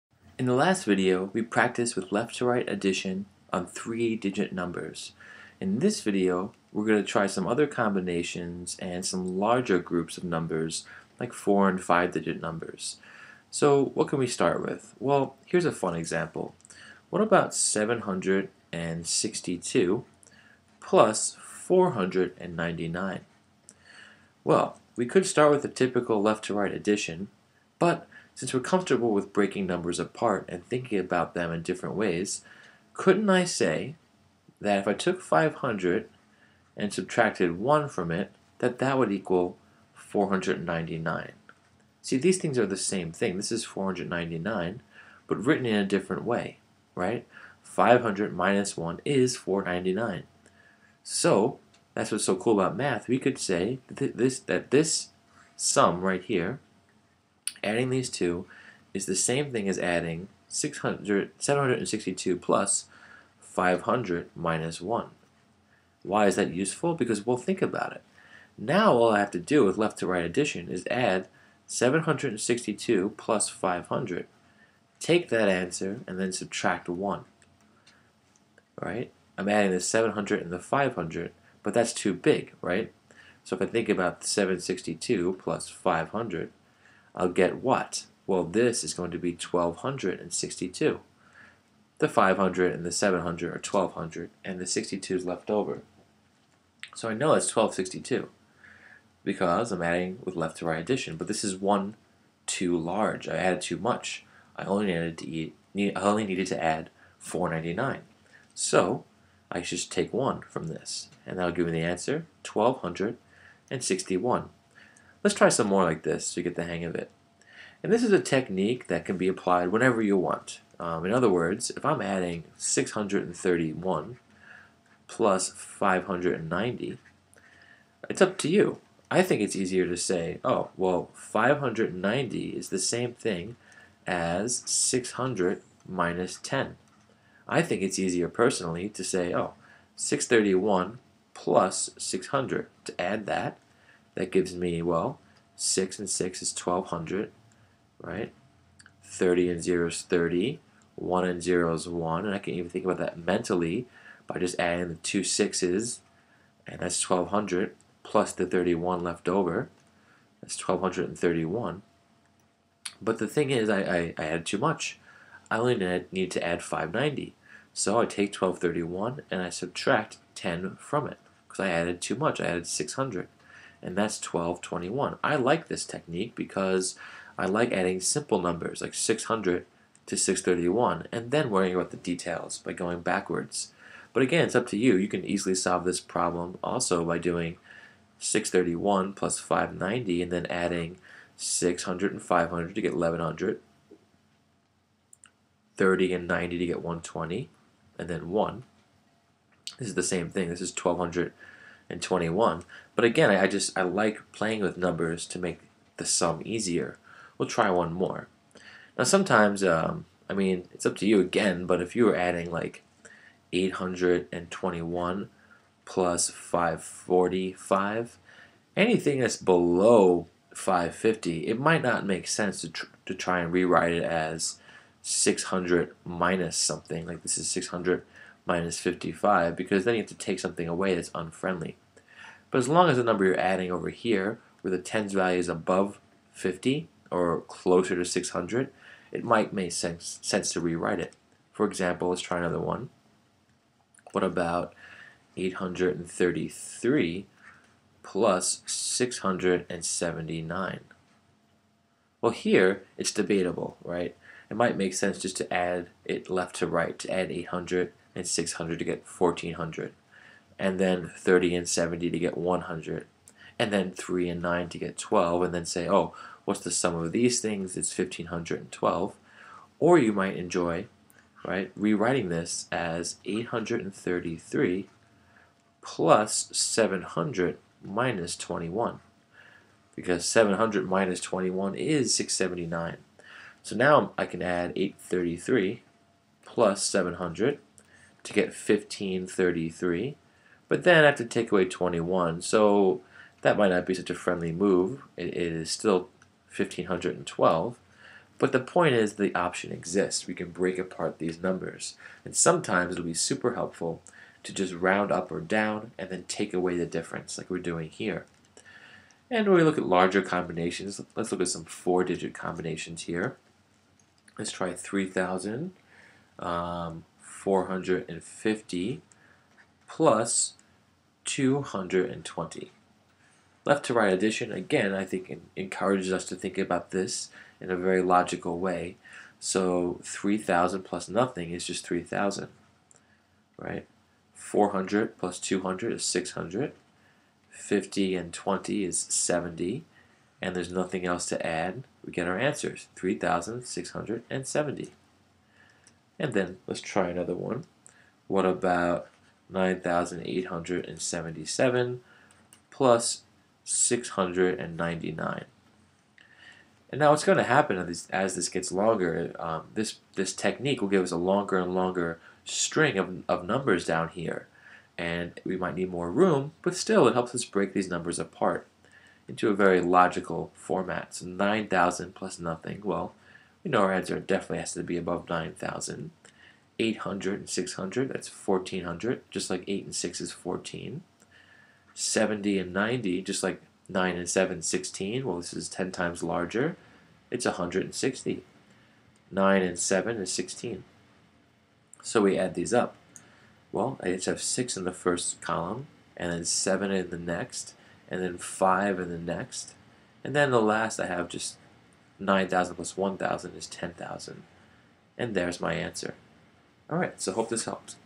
Left to Right Addition Practice
Problems with left to right addition that you 3 and 4 digit combinations and more